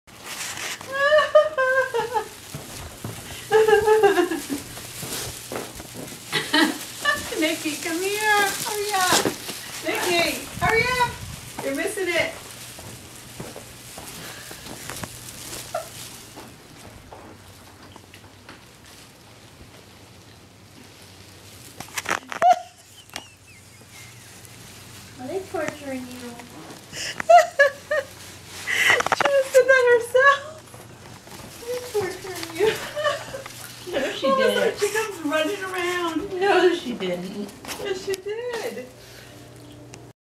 Munchkin was snooping around and got caught in bag
That is Nikki's mom laughing in the background.